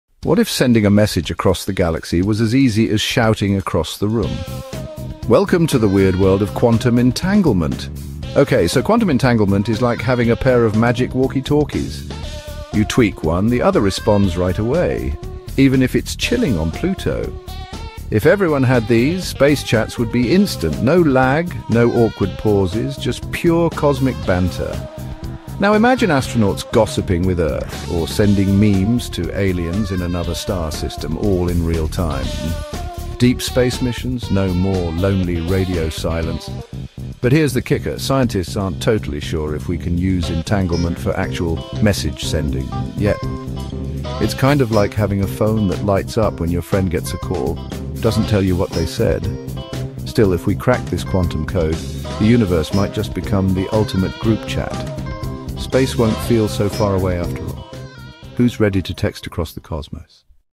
everything in the whole wide of existences is connected :Quantum Entanglement
Quantum entanglement is a phenomenon where particles that have interacted with each other become connected in such a way that the state of one particle is directly related to the state of another, no matter the distance between them.